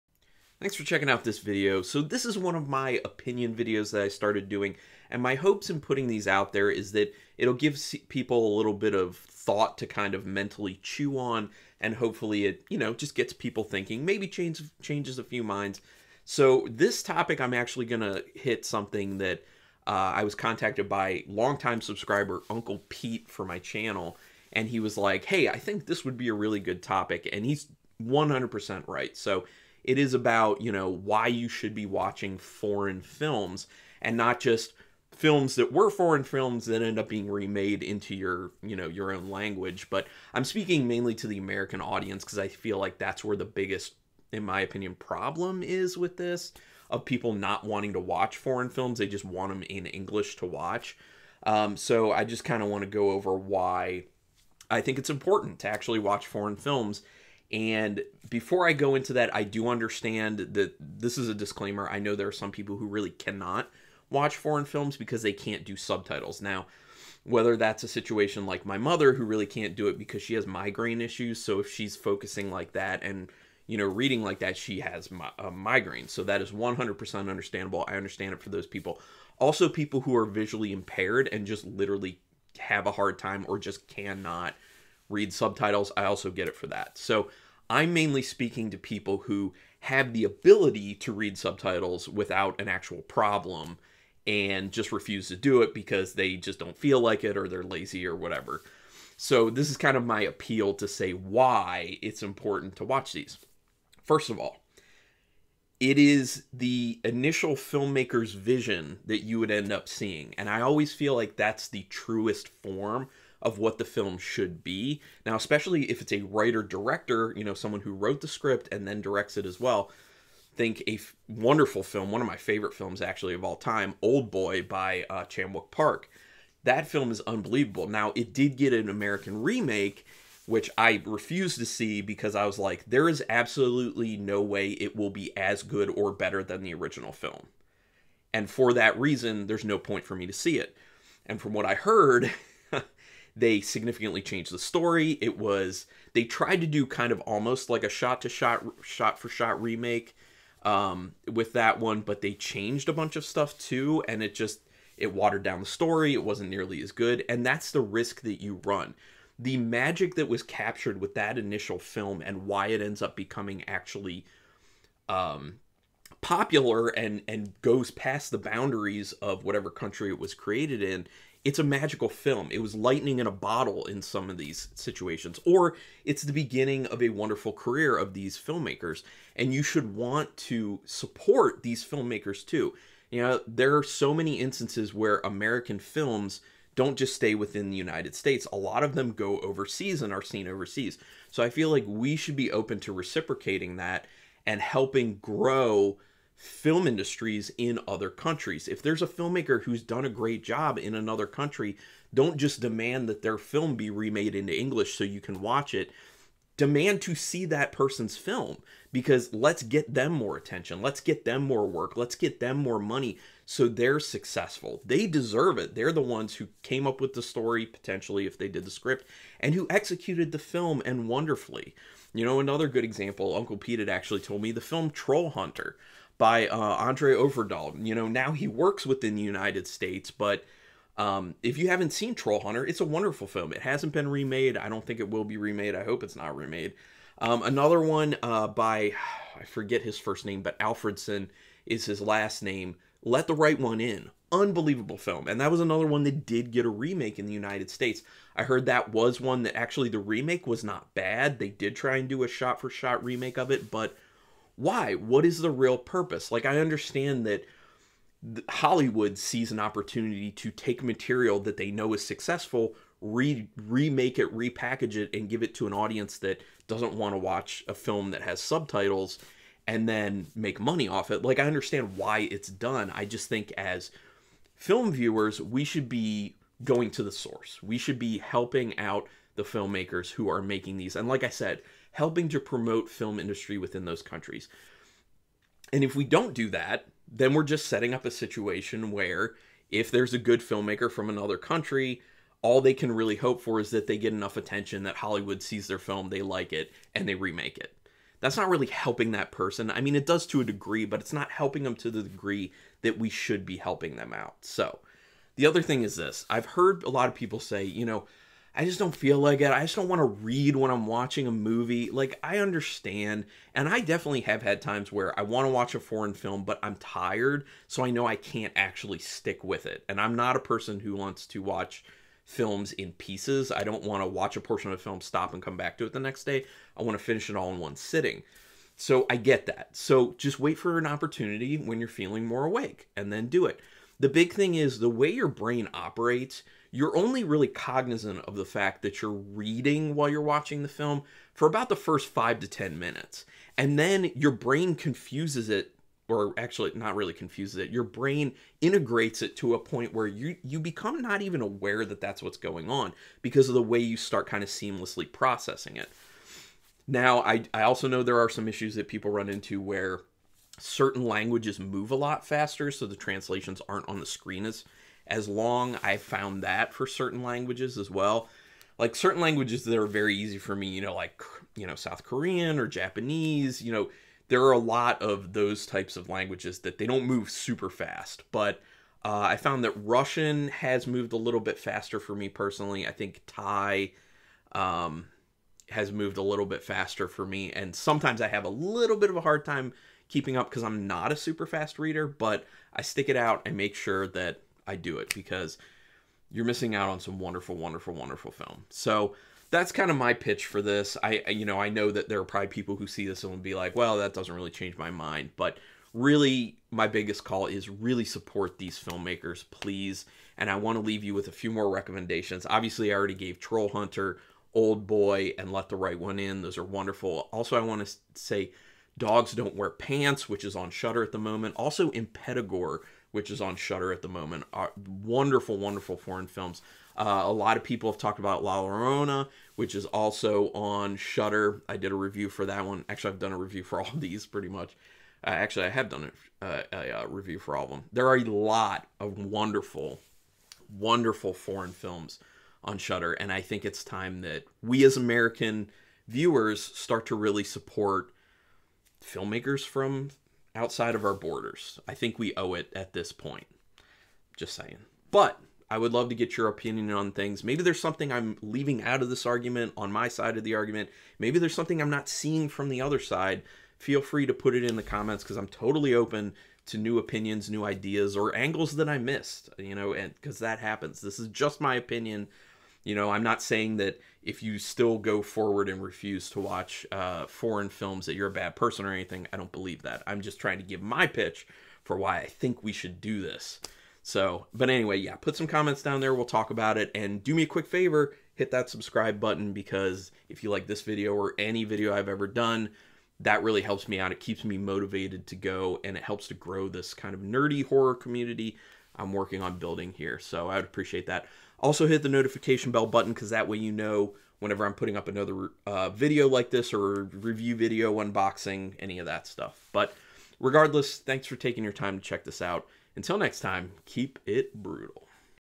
Why Watching Foreign Films Is Important
Carlin gives his opinion on why everyone should give foreign films a chance.